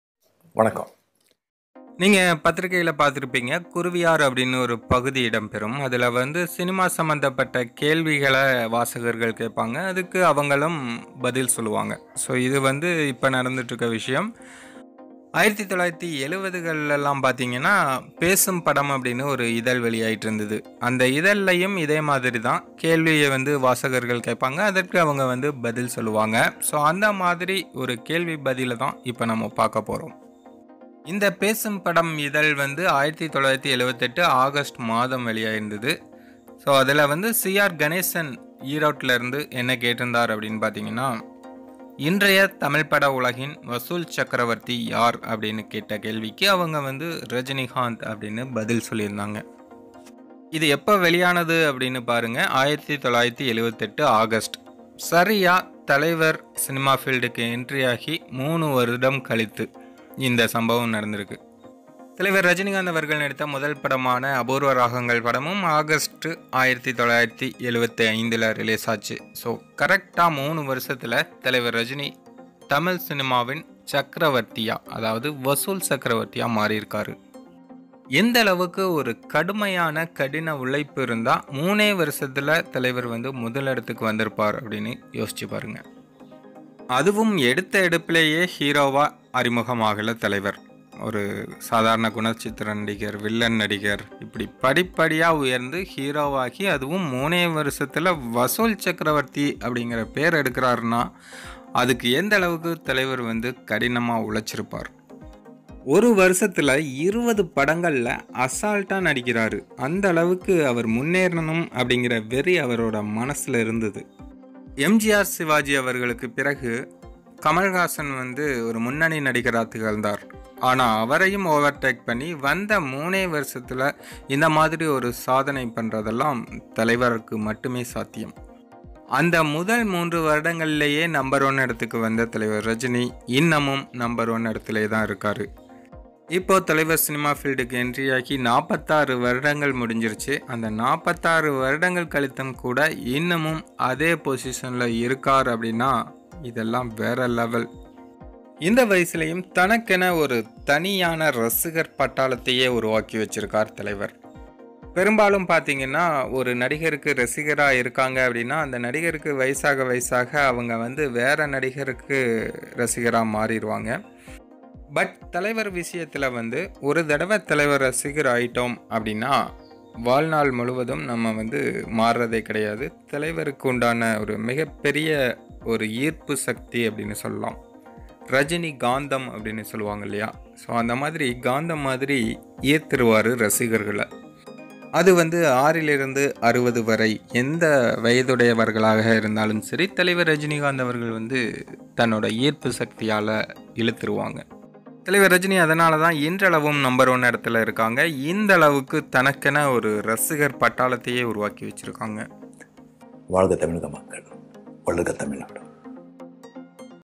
சினிமாவில் வசூல் சக்ரவர்த்தி யார்? பேசும்படம் என்ற மாத இதழில் வெளியான கேள்வி பதில்! வருடம் 1978
😎KAALAWIN KAAVALARGAL😎

Thalaivar Forever🤘